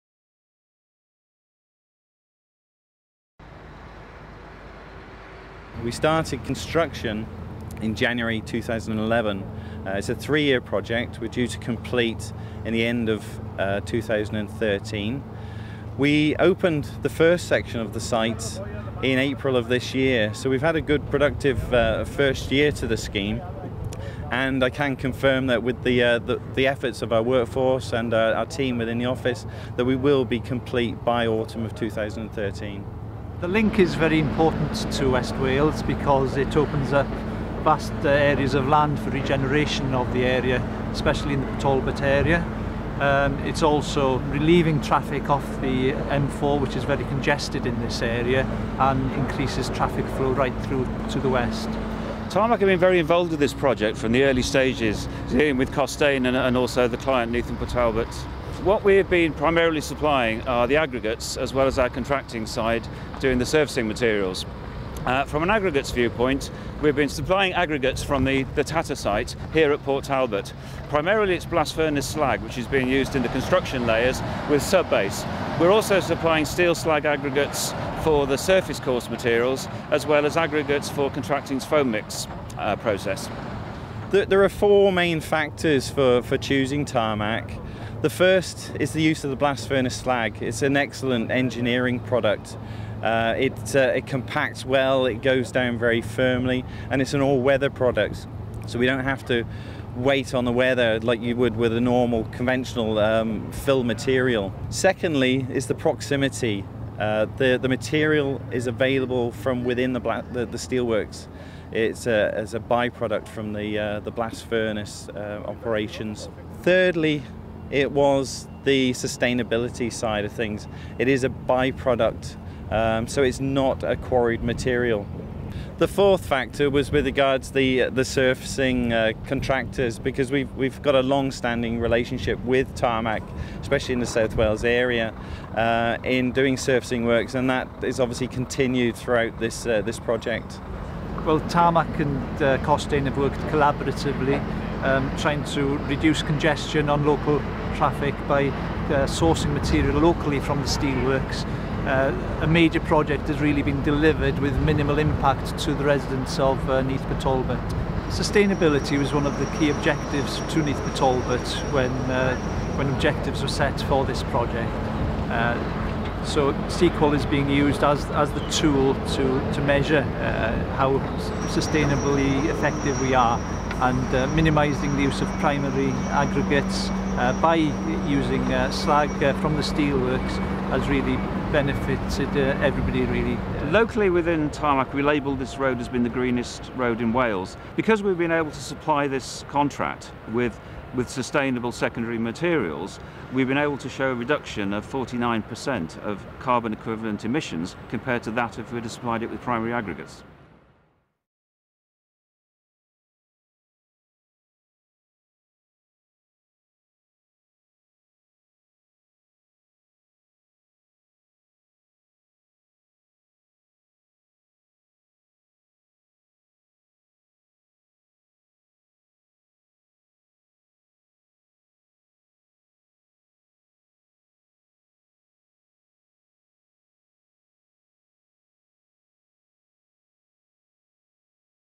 Tarmac Blast Furnace Slag Aggregates for Port Talbot
Hear why Costain chose Tarmac's Blast Furnace Slag aggregates for their Port Talbot project.  By using  sustainable secondary materials Costain have been able to show reduction of 49% of carbon footprint emissions compared to primary aggregates.